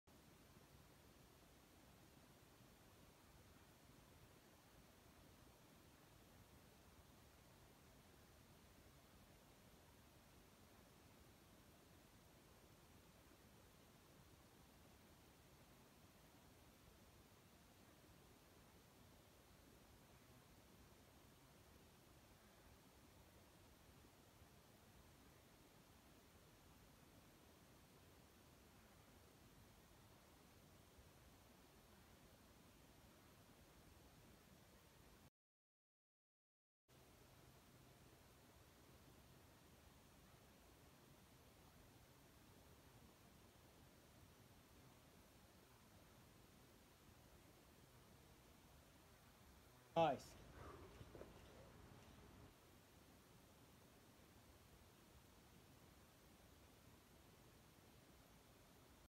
Cooper’s Rock Bouldering: Ship’s Prow, V5
Ship’s Prow, V5 at Upper Rock City at Cooper’s Rock in West Virginia.